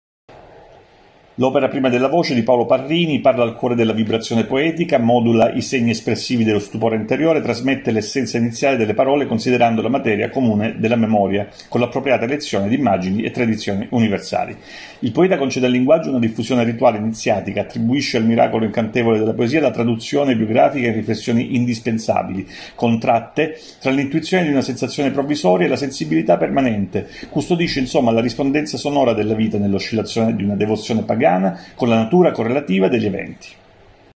Prima della voce di Paolo Parrini (Samuele Editore)
L'opera “Prima della voce” di Paolo Parrini (Samuele Editore - Collana Callisto, 2021 pp. 70 € 12.00) parla al cuore della vibrazione poetica, modula i segni espressivi dello stupore interiore, trasmette l'essenza iniziale delle parole considerando la materia comune della memoria con l'appropriata elezione d'immagini e tradizioni universali. Il poeta concede al linguaggio una diffusione rituale, iniziatica, attribuisce al miracolo incantevole della poesia la traduzione biografica in riflessioni indispensabili, contratte tra l'intuizione di una sensazione provvisoria e la sensibilità permanente, custodisce la rispondenza sonora della vita nell'oscillazione di una devozione pagana con la natura correlativa degli eventi. Prolunga il senso sottile e delicato della relazione estetica con l'unità dell'esperienza sensibile, osserva il presentimento acuto della visione del mondo e dei suoi struggenti accordi, traccia il rilievo emerso delle emozioni, distingue la cavità difensiva dell'ispirazione come il salvifico territorio delle occasioni e della verità. Il percorso elegiaco incrocia la scheggia indicativa nell'intreccio dei ricordi, l'intensità dello sguardo quotidiano sulla consistenza saggia della realtà. “Prima della voce” rintraccia la grammatica e la ricostruzione dei significati affettivi, recupera il dialogo spirituale trasferendo nella rappresentazione delle fotografie artistiche, contenute nel libro, la contemplazione della bellezza, riscatta la percezione delle impressioni che il disincanto ha estinto intorno alla nostra esistenza. Paolo Parrini riacquista la possibilità di vivere i legami con la naturale capacità di ascoltare e capire le proprie passioni, accoglie la cura dei sentimenti, concentra il raccoglimento religioso delle attese nei labirinti dei propri desideri, salvaguarda il sincero legame con le proprie promesse, affermando l'estensione di un'esecuzione lirica obiettiva, l'elevazione di un'epifania meravigliata, in comunione con un equilibrio riportato in  luce oltre la discordanza oscura del vivere. Risana l'intermittente dimensione del tempo e la direzione di appartenenza ai propri versi, ricompone le incertezze nell'esercizio stilistico di conquista dell'amore e di perizia dell'inquietudine, abita il luogo esteso dell'anima, ospita intenzioni e metafore della quiete. Il ritmo dei testi celebra la visione dinamica della pagina, come spazio e corpo degli elementi letterari, il carattere sacro e sensuale di una conversazione insistentemente scampata alla dimenticanza. La poesia di Paolo Parrini riconduce sulla soglia di un avvenimento, traduce il realizzarsi scrupoloso della successione del rumore e della sospensione, nel calpestio dei passi della vita,  definisce una voce segreta e ritrovata, estende la cortina della fragilità nell'infinito riflesso dell'estremità esistenziale, percorre le venature, la condensazione e  l'evaporazione dell'assenza. Fonda la sua dottrina nel respiro del miracolo sacro e familiare della tenerezza, nell'impalpabile sensualità, annoda il tessuto evocativo dei luoghi inattesi, escludendo il debito della parola alla deviazione del silenzio: “La memoria non è ciò che ricordiamo, ma ciò che ci ricorda. La memoria è un presente che non finisce mai di passare.” Octavio Paz

Intervento di Rita Bompadre - Centro di Lettura “Arturo Piatti”